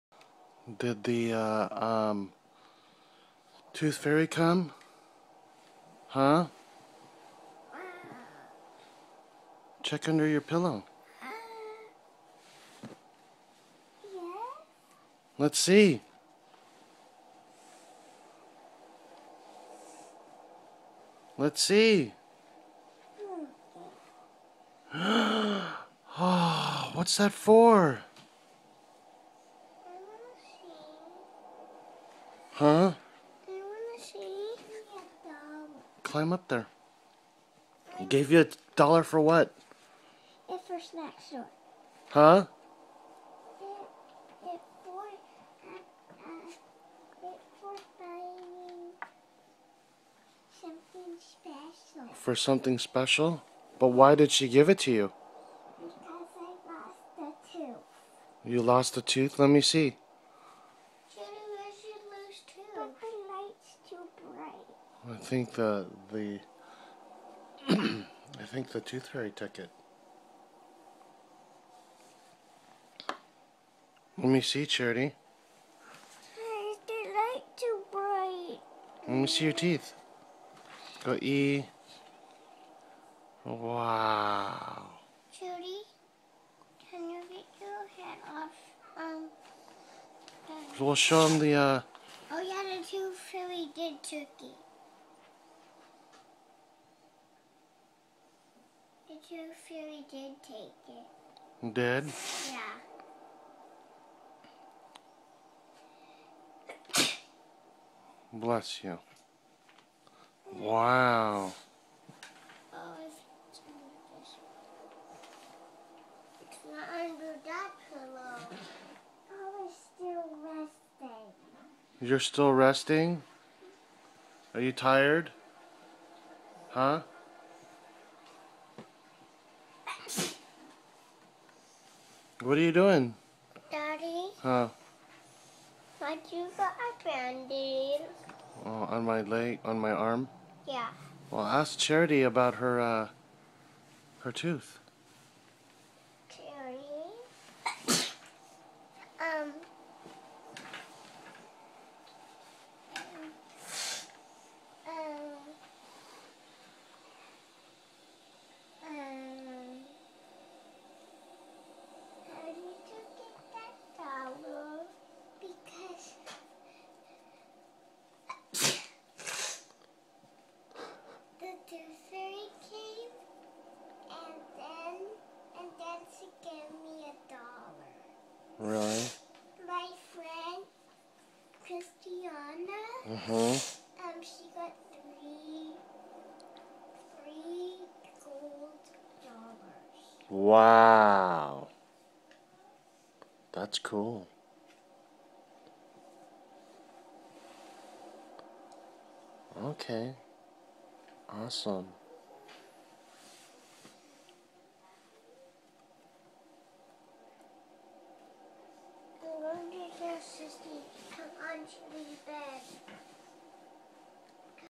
Tooth fairy for Charity 12.2014
Tooth fairy came for Charity and her second lost tooth 12.2014